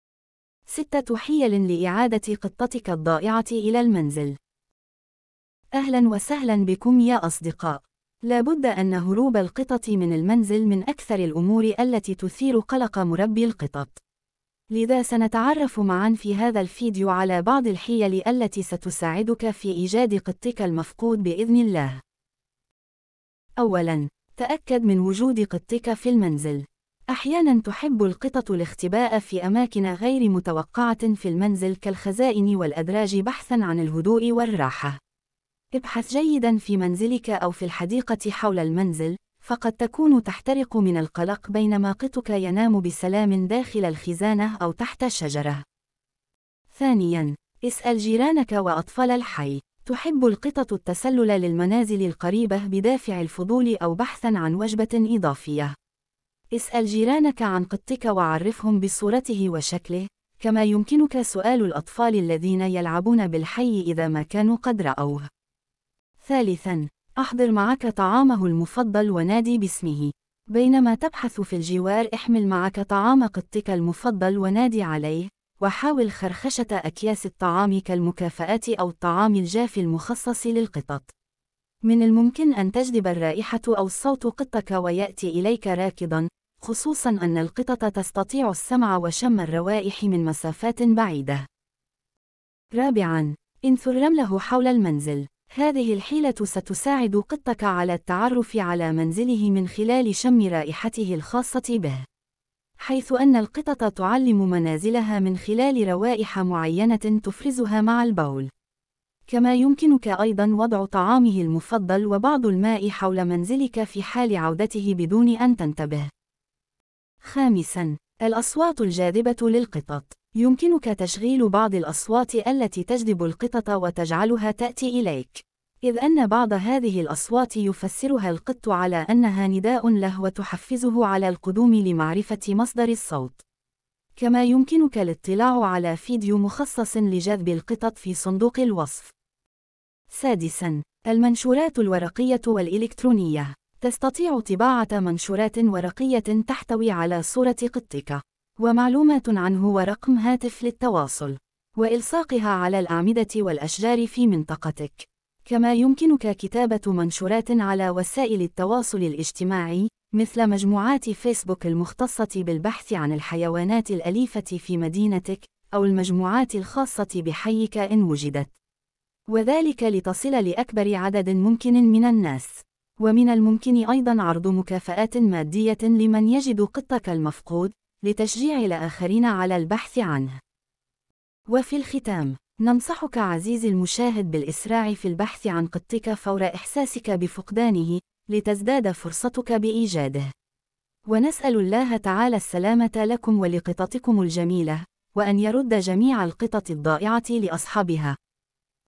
6 حيل لإعادة قطتك الضائعة إلى المنزل
أصوات تجذب القطط وتجعلها تأتي إليك:
تعرف إلى أنجح 6 حيل لإعادة قطتك الضائعة إلى المنزل..
لا بد أن فقدان القطط كابوس يؤرق الكثيرين من مربي القطط ولذلك نقدم لكم في هذا الفيديو أهم الحيل التي ستساعدك في إيجاد قطتك المفقودة وإعادتها إلى المنزل بسلام بإذن الله..